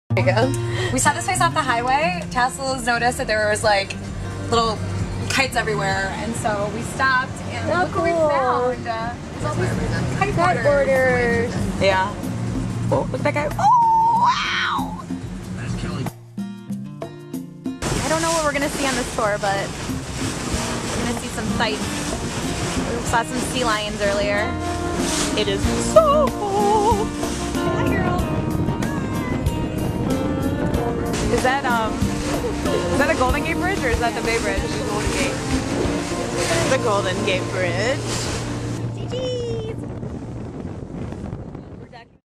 Sight seeing in San Fran with the Hanes Comfort Force
Watch as the Hanes Comfort Force goes sightseeing in San Francisco